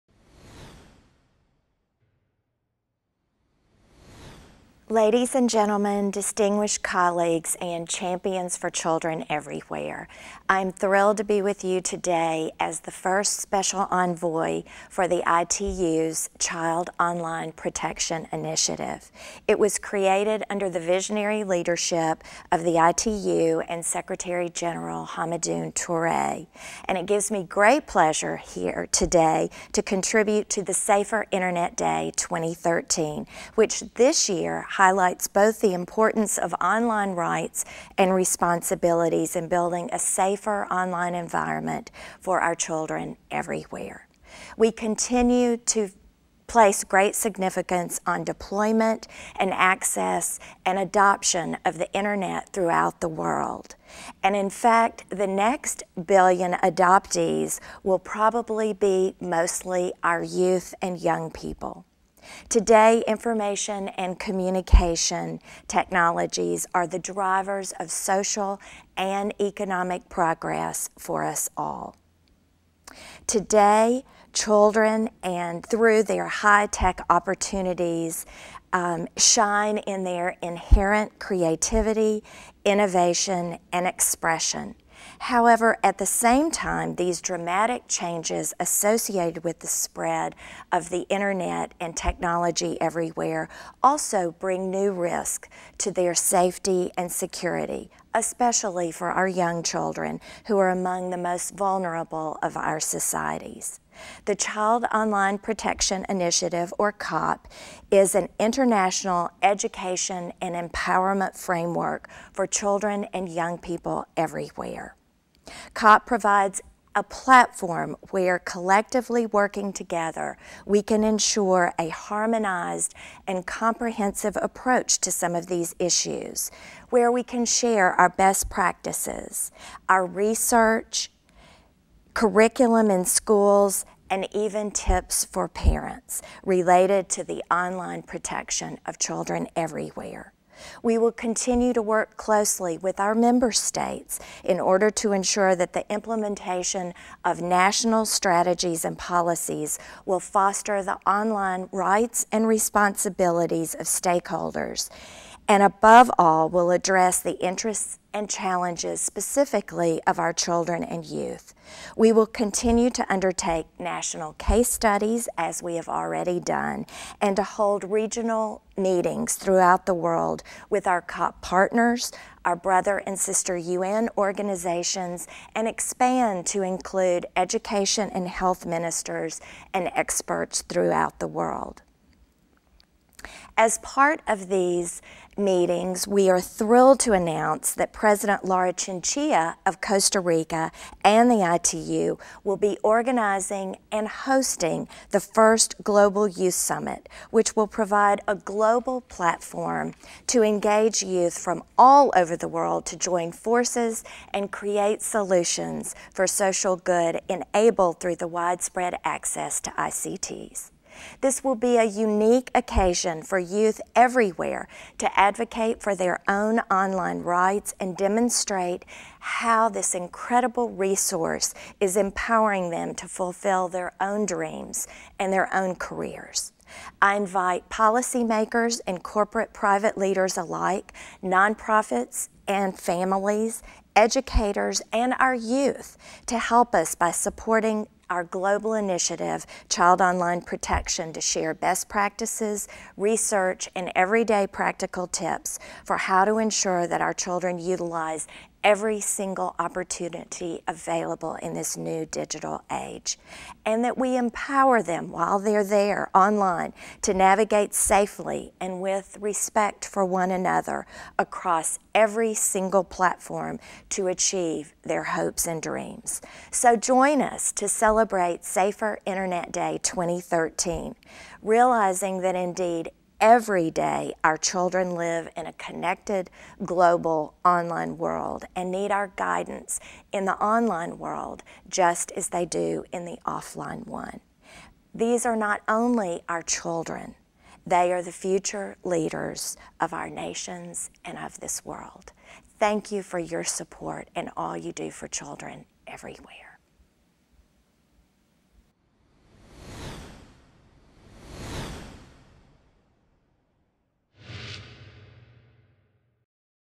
SAFER INTERNET DAY 2013 VIDEO MESSAGE: Deborah Taylor Tate, ITU Special Envoy
ITU Video Message for Safer Internet Day 2013 from Deborah Taylor Tate, ITU Special Envoy.

On the occasion of the Safer Internet Day 2013, ITU hopes that today will not just be a one-day celebration. Indeed, it is our commitment to make 2013 a year-long celebration -- ensuring that by working together we can build a really effective global framework to protect children and young people online.

Help ITU by joining ITU's Child Online Protection initiative -- and help contribute to making cyberspace peaceful, and protecting our future, and our children's future.